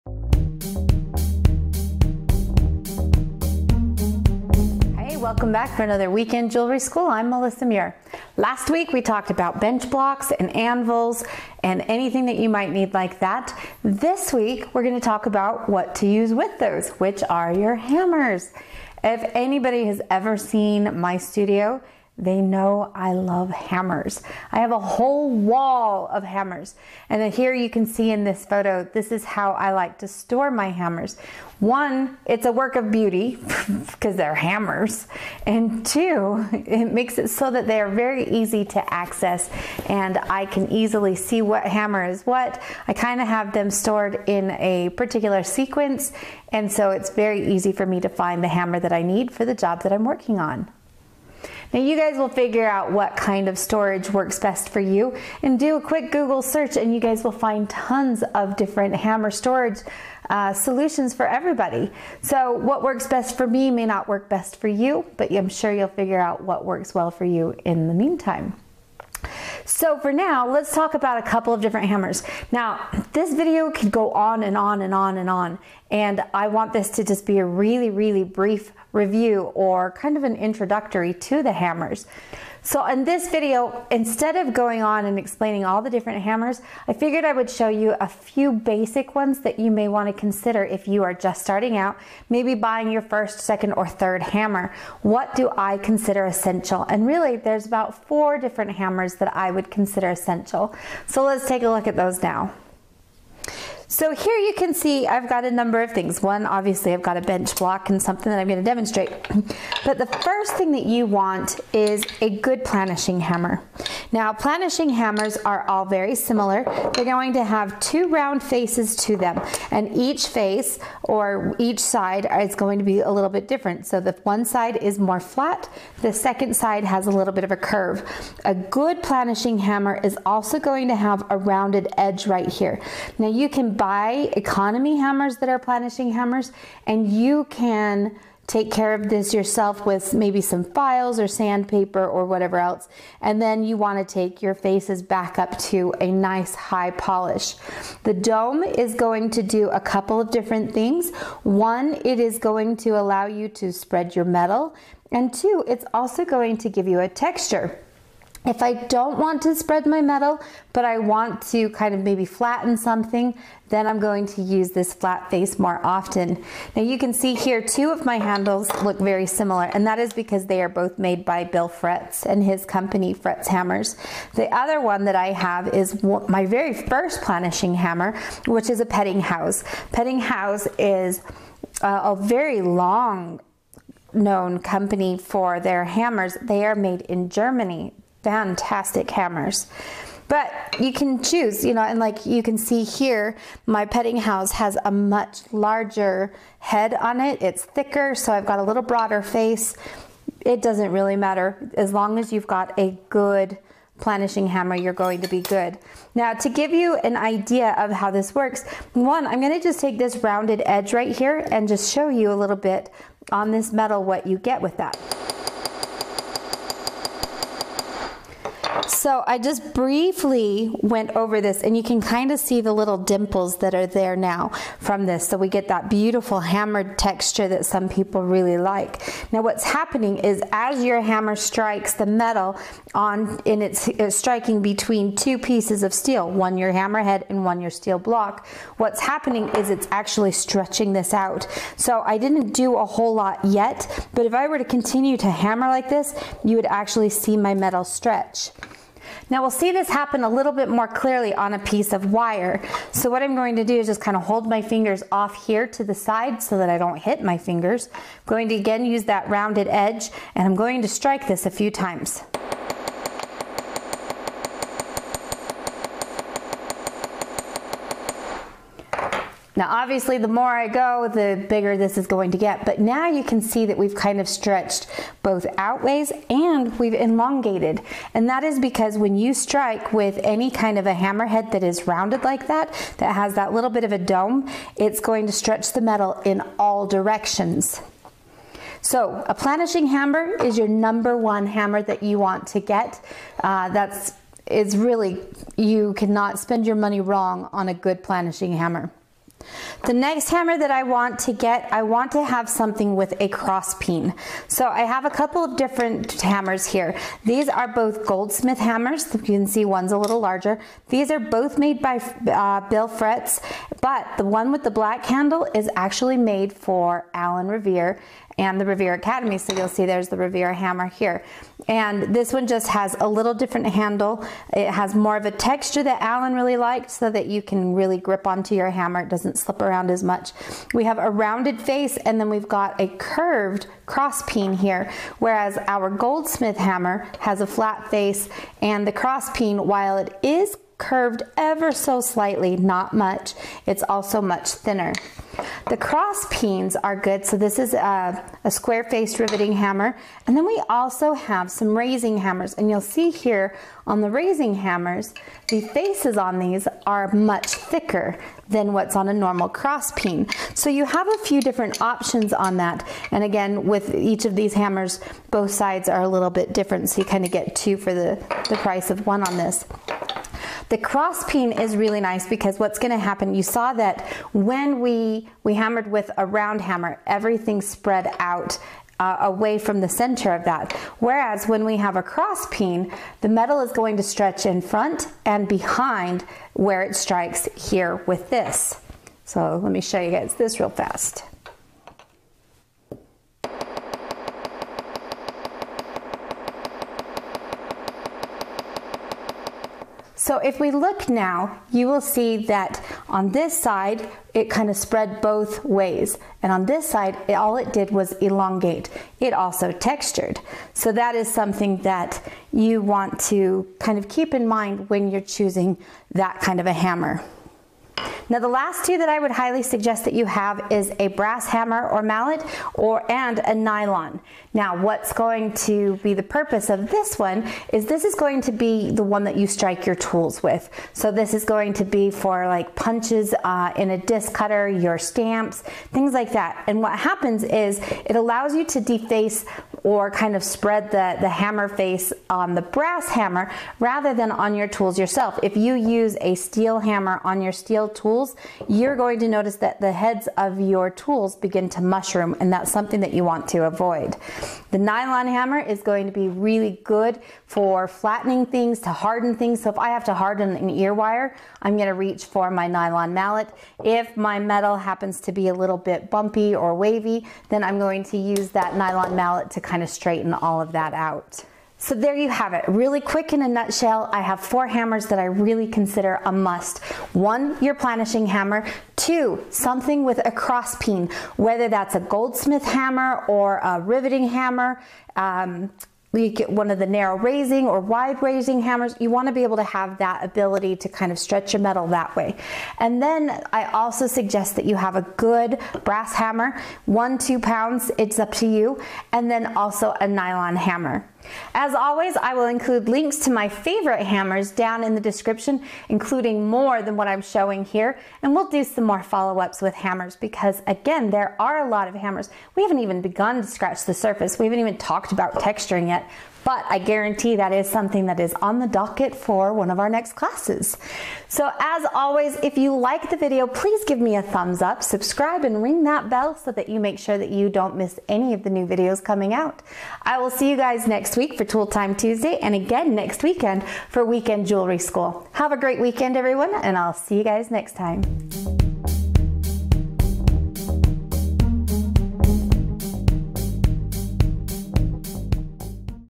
Weekend Jewelry School - Session 8 - Intro to Hammers
Last week we talked about bench blocks and anvils. This week let's talk about the hammers to use with them. I will share what I consider to be four essential hammers in your studio.